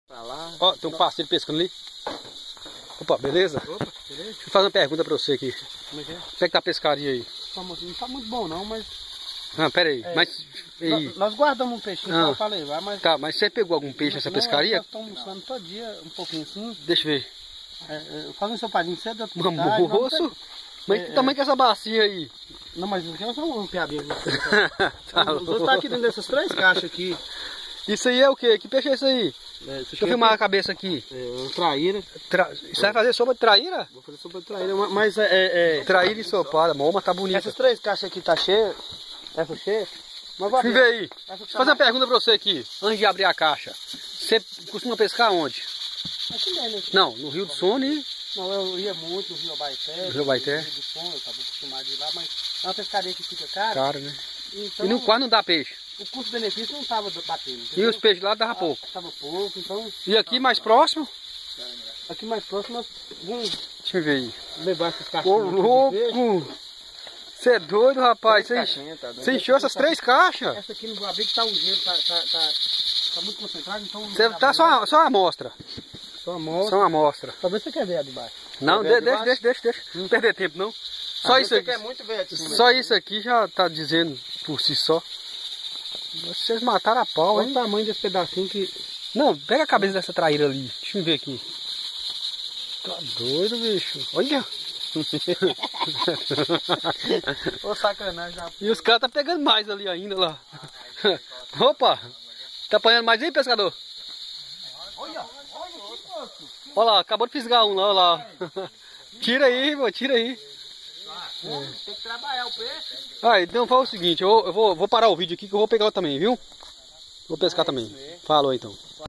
Rio jequitaí,saiu até uns peixinhos
essa pescaria foi uma pescaria que nós fizemos de última hora. não foi necessário ir para muito longe, foi bem próximo de onde moramos. até que saiu uns peixes até bom.